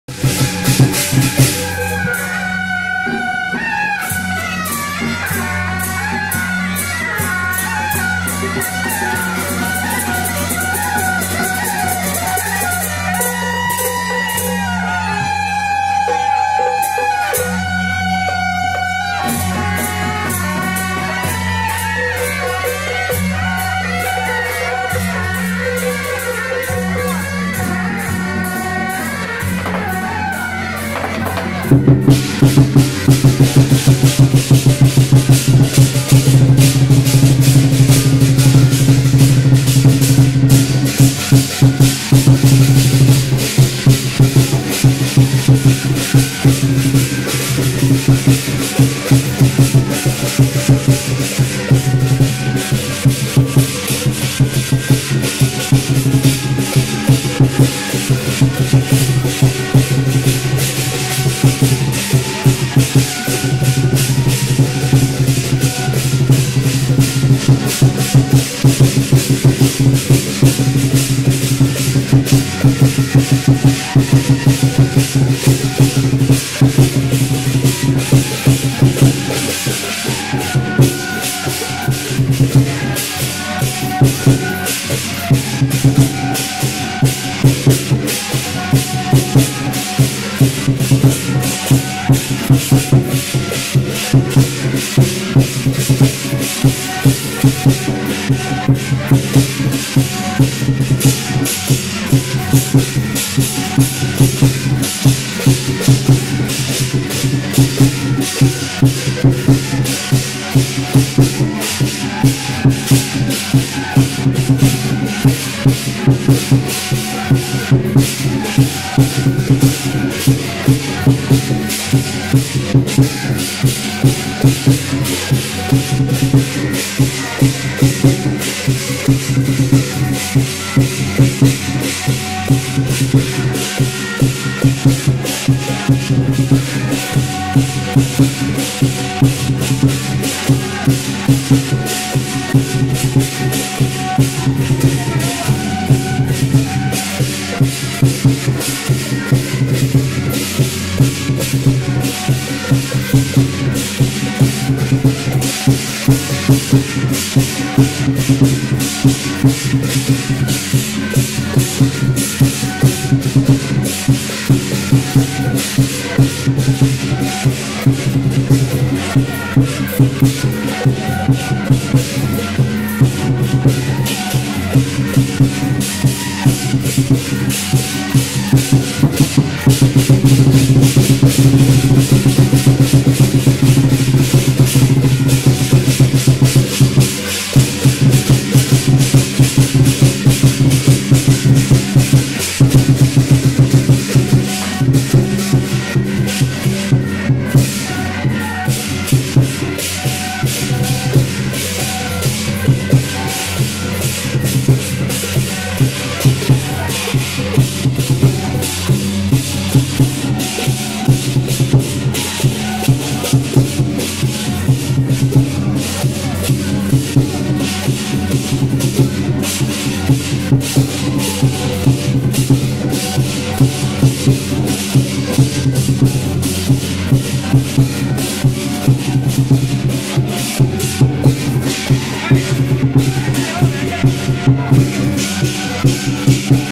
107中和顯武堂天官武財神三年圓科南巡進香回駕遶境【永和神將聯誼會+板橋玄威堂-神將團】
永和神將聯誼會+板橋玄威堂-神將團